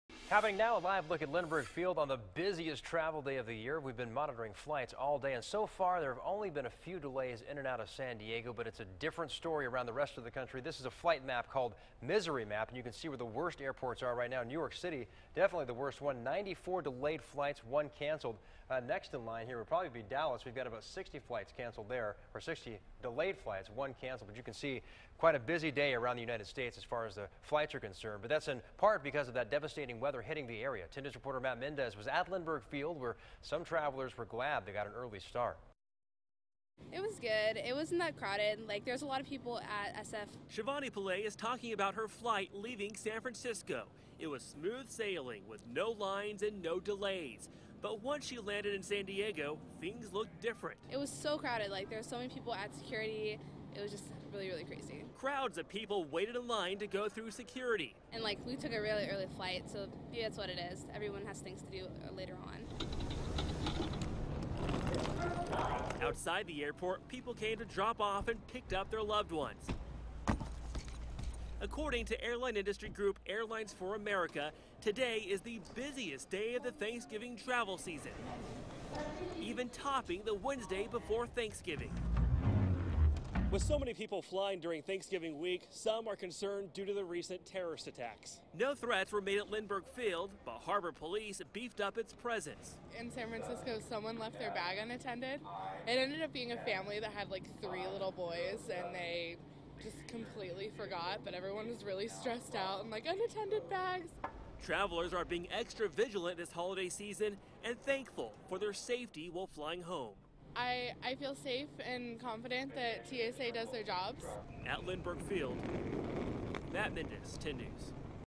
People return home from Thanksgiving vacation
Sunday after Thanksgiving is one of the busiest days for air travel.